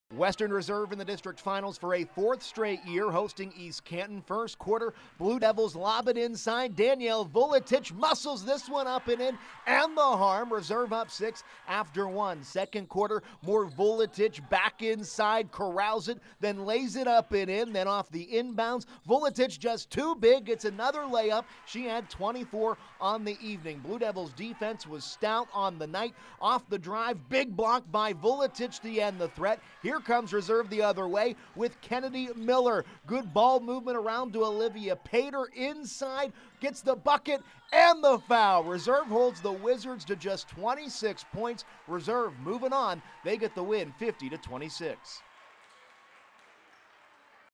Highlights: Western Reserve earn district crown with win over East Canton
Western Reserve advances to face rival McDonald on Thursday in the Regional Semifinals